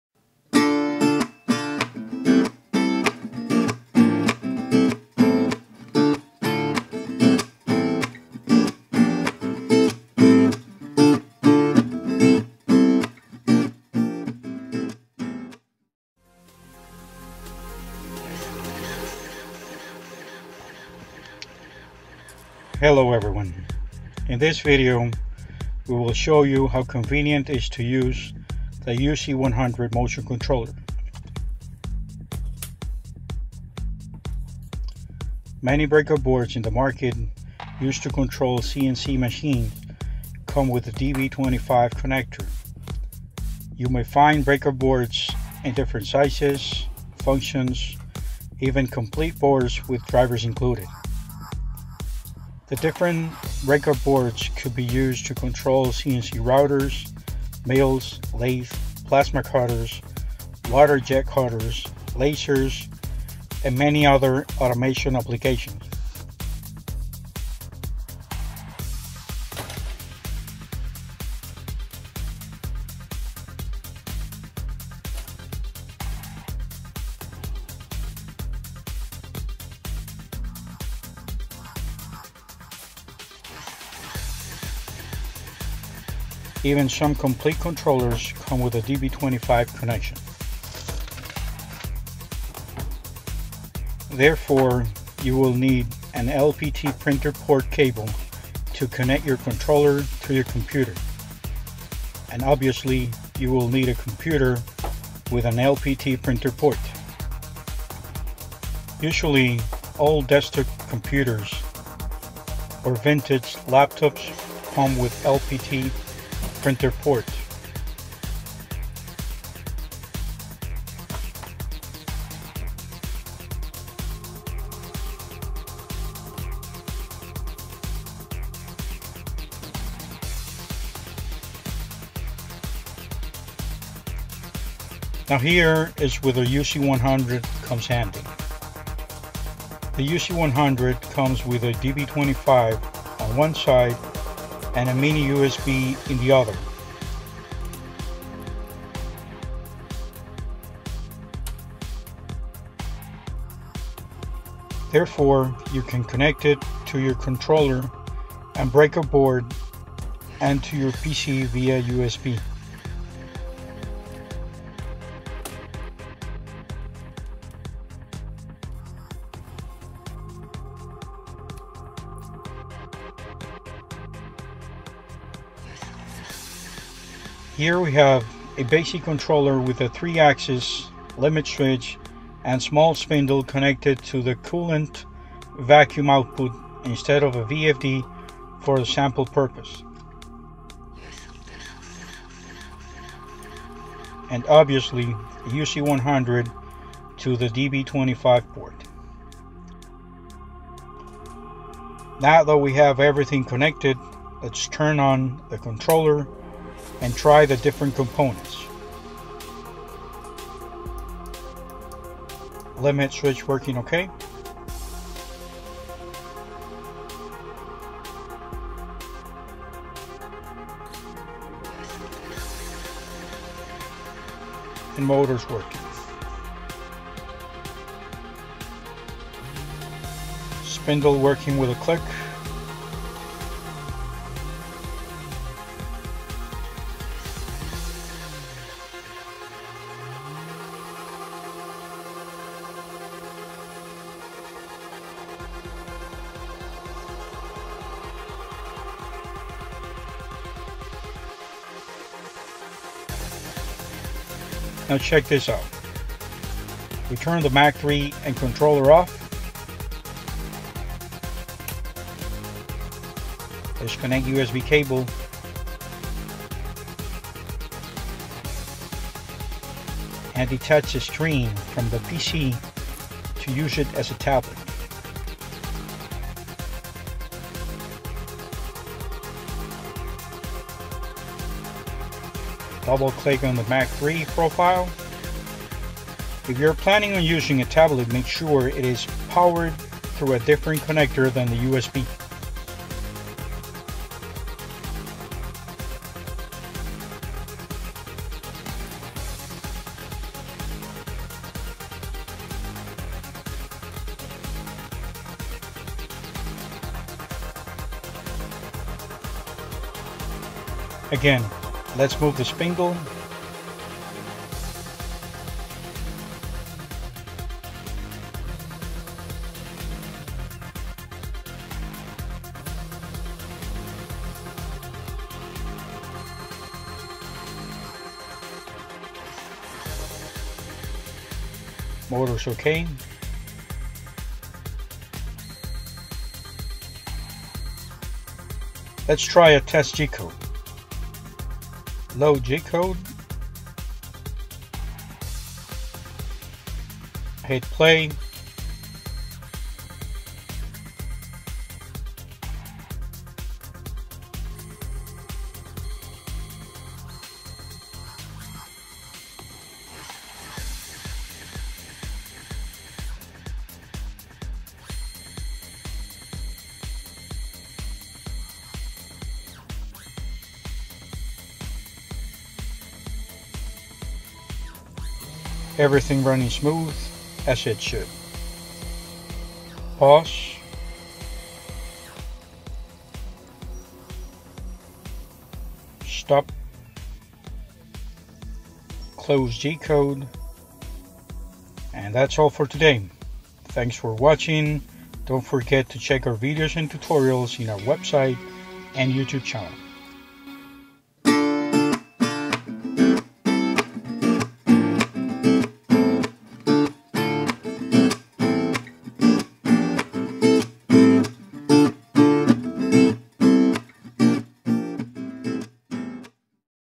why uc100
Why using UC100 motion controller is a great way to replace old desktop computers and vintage laptops using LPT parallel port with new up to date computer and windows versions making easy and simple to run your cnc controller and cnc machine by connecting to a contemporary USB technology.